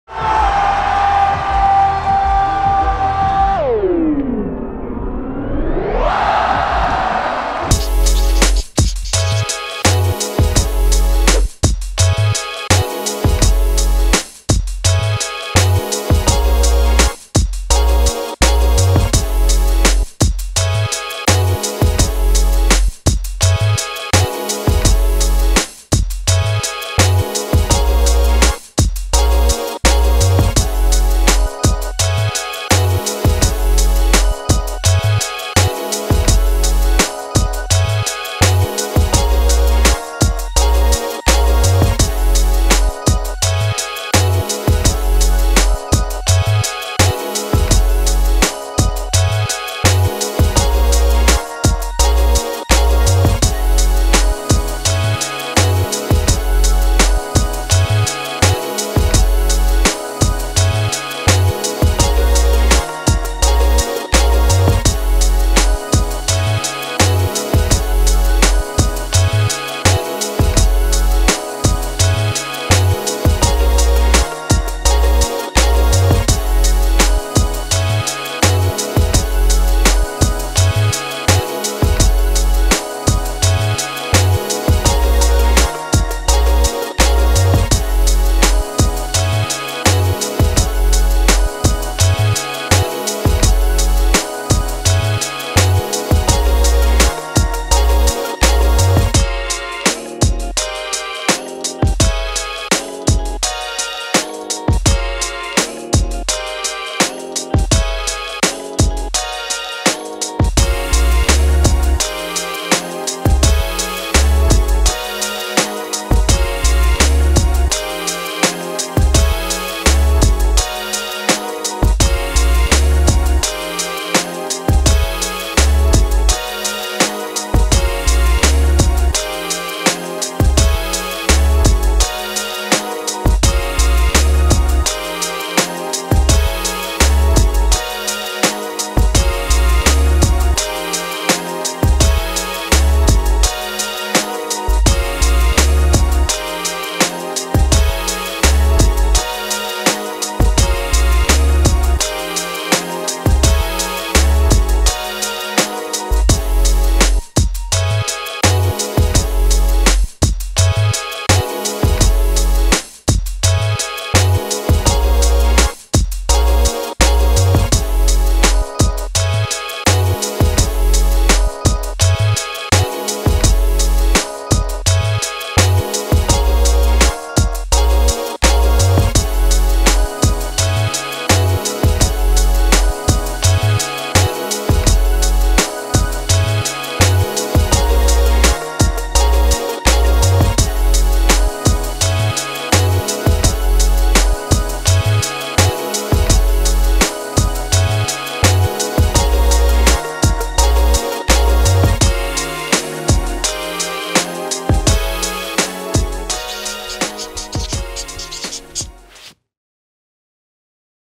2019 FISE: Montpellier - The Bangers
All of the bangers from men's park finals at FISE: Montpellier 2019 featuring Justin Dowell, Brandon Loupos, Kyle Baldock, Irek Rizaev, Logan Martin, Nick Bruce, and more!